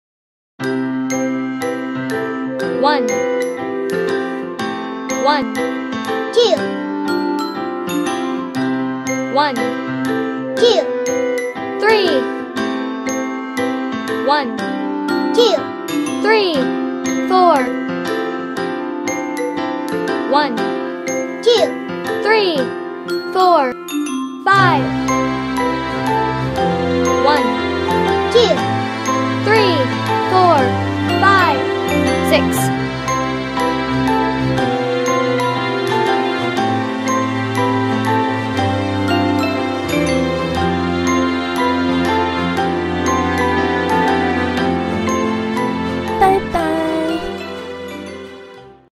Counting Minions. How many are there?
Who doesn't like Minions... Because of that PuzzledCat decided to count them. Learn numbers with us.